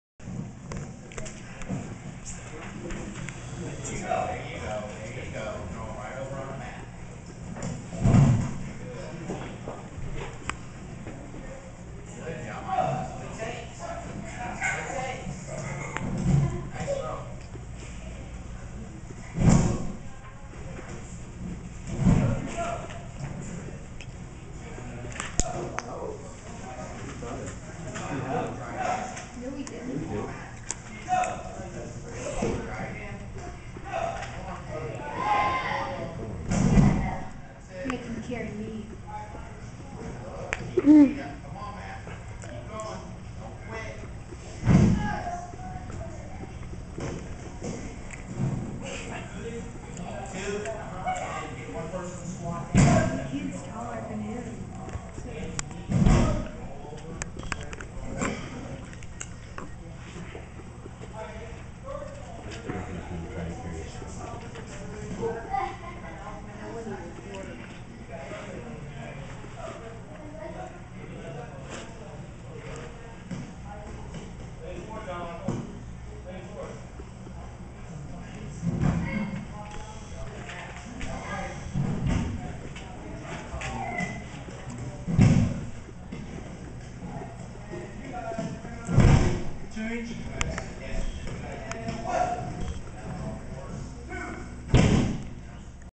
Wesley Hanson Training
me training to fight